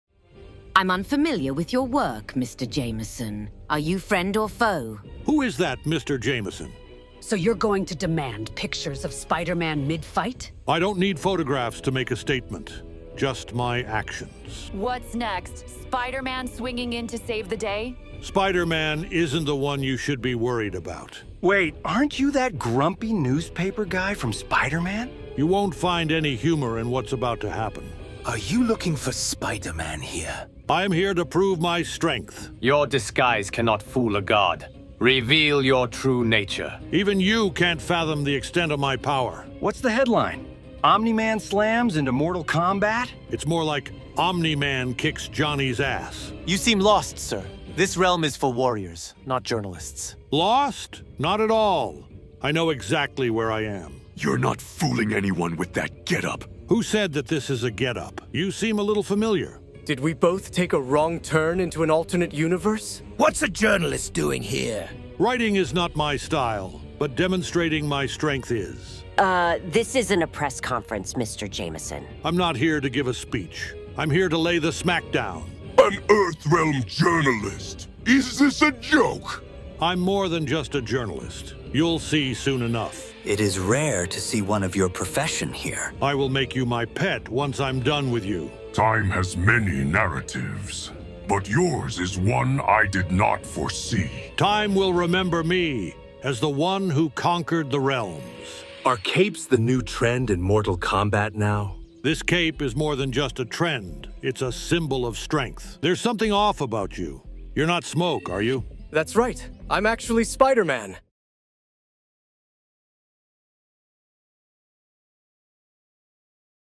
Everyone Mistakes Omni-Man for Jonah Jameson
They both look strikingly similar. Mortal Kombat characters are confused because now everyone thinks he's here to get Spider-Man...

Please bear in mind that this is fan-made and not real dialogues from the actual game.

cheers! :)